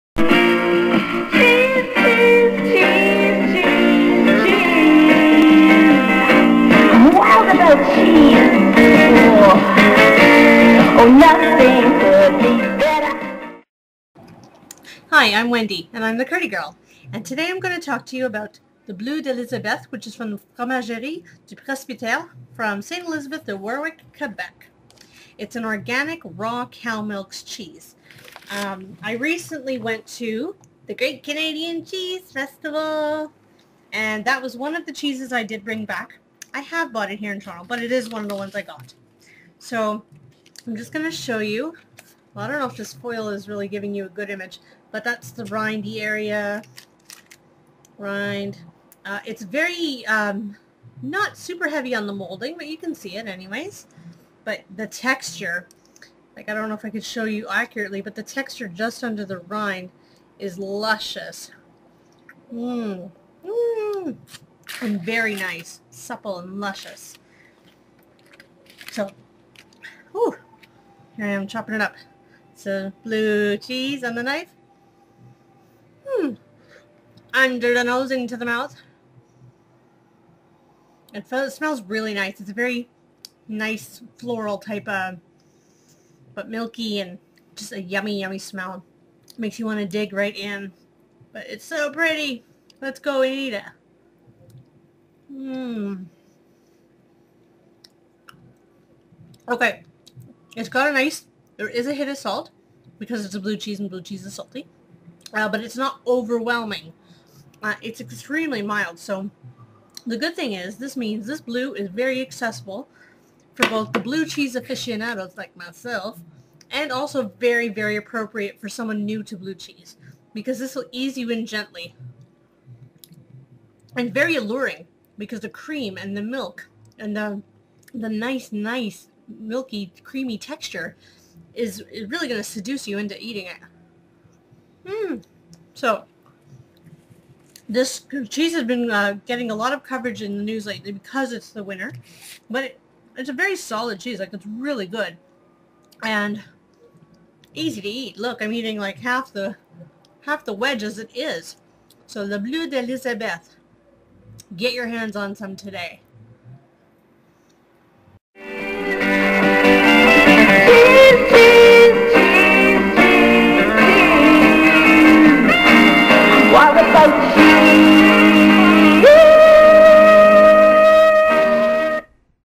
CurdyGirl Bleu D'Elizabeth
CurdyGirl eats Le Bleu d'Élizabeth a raw, organic cow's milk blue cheese from Quebec. The producer of this cheese, Fromagerie du Presbytère (Quebec), has made a lot of stellar award winning cheeses; this cheese won the top prize for blue cheese in this competition.  Creamy luscious goodness awaits!